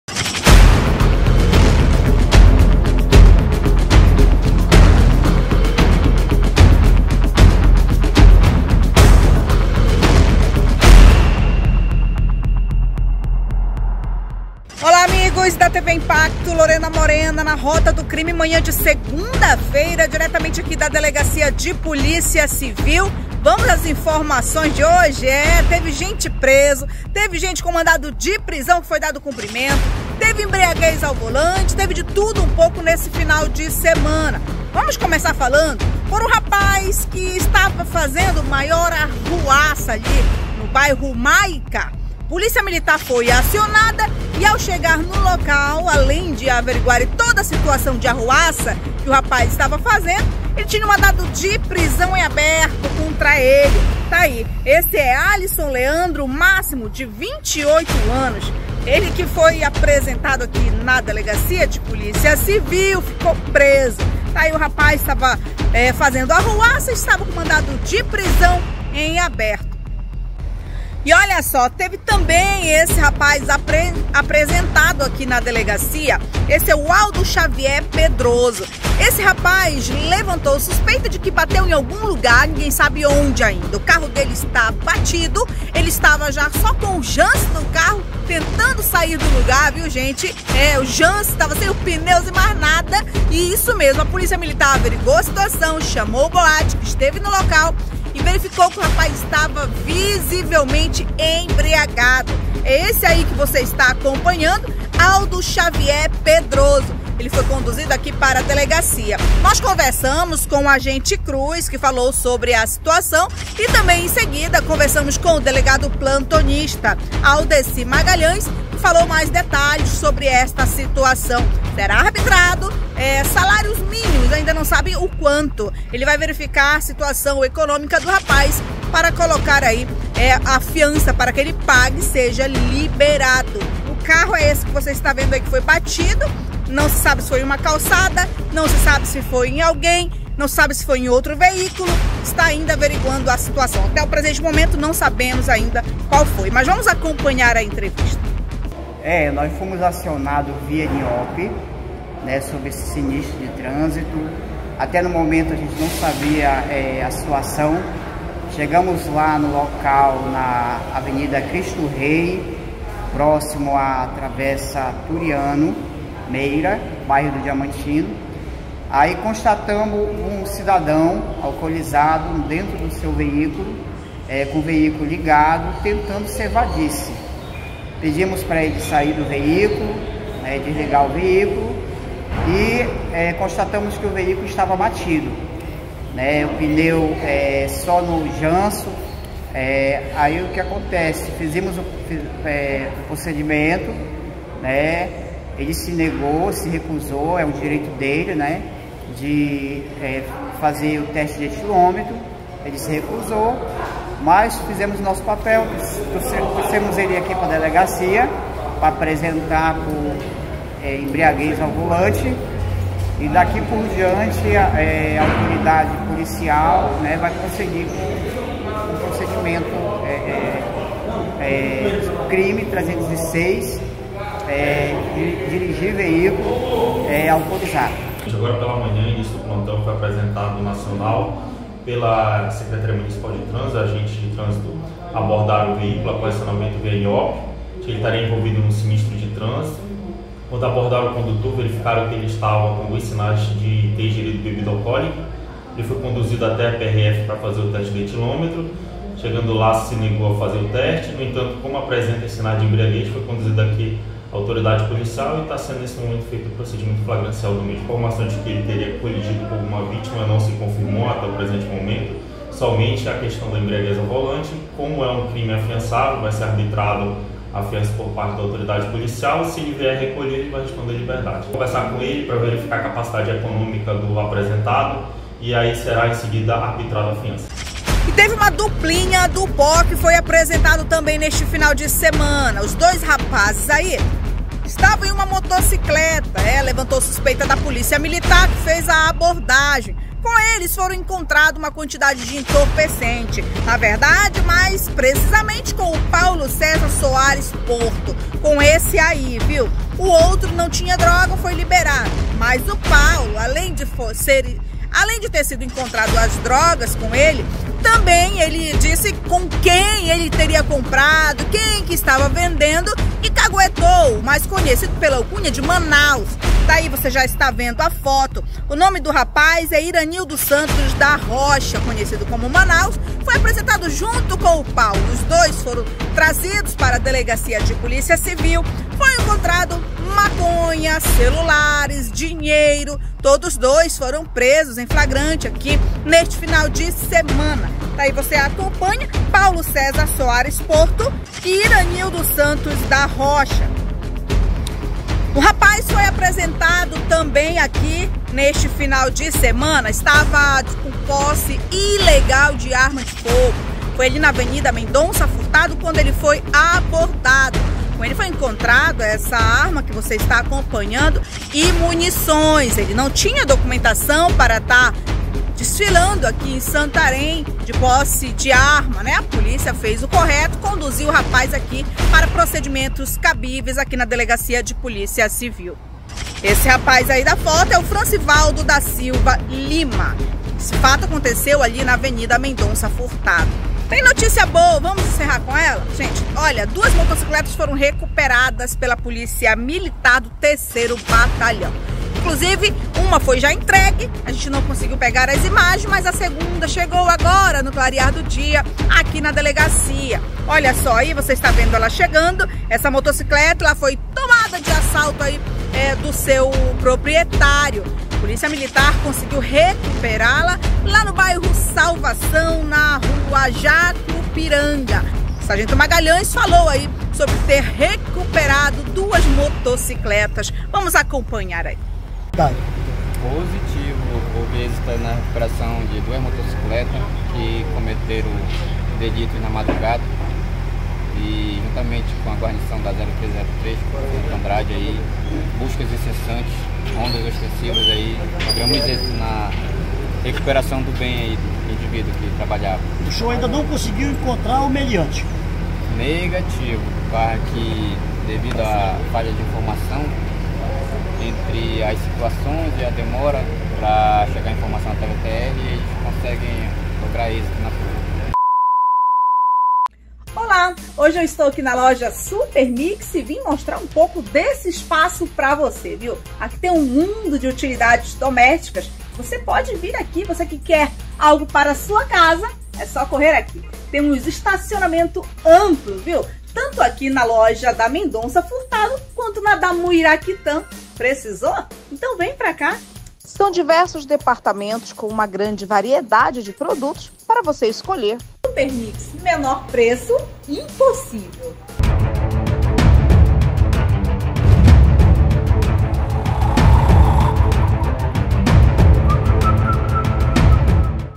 🚓 DESTAQUES: EMBRIAGUEZ, MANDADO DE PRISÃO, POSSE ILEGAL DE ARMA DE FOGO E TRÁFICO
✔️Motorista com carro batido foi abordado pela PM que acionou BOAT. Foram constatados visíveis sinais de embriaguez. A suspeita é que o carro colidiu em algo que o condutor não soube explicar, diante dos fatos foi preso em flagrante. Falaram a respeito da situação o delegado e o agente de trânsito. Acompanhe!

✔️Homem foi apresentado na seccional de PC portando arma de fogo na Av. Mendonça Furtado.  Arma e munições foram apreendidas.

✔️Dupla do pó foi presa pela PM. Maconha, celulares e dinheiro foram apreendidos com os indivíduos.

✔️Duas motos que haviam sido roubadas foram recuperadas pela PM do 3º BPM. Veja!